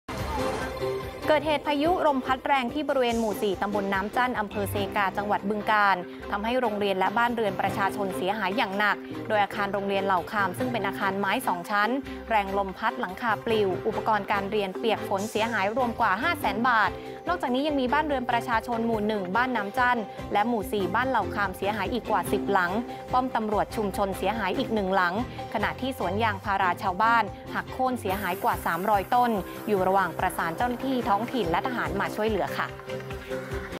พายุพัดถล่มบึงกาฬบ้านเรือนประชาชนและโรงเรียนเสียหาย
พายุพัดถล่มบริเวณหมู่ 4 ตำบลน้ำจั้น อำเภอเซกา จังหวัดบึงกาฬ ทำให้โรงเรียนและบ้านเรือนประชาชนเสียหายหนัก

1.สารคดีเฉลิมพระเกียรติ
2.พระราชกรณียกิจ-พระมหากรุณาธิคุณ
3.คนไทยหัวใจ♥รักในหลวง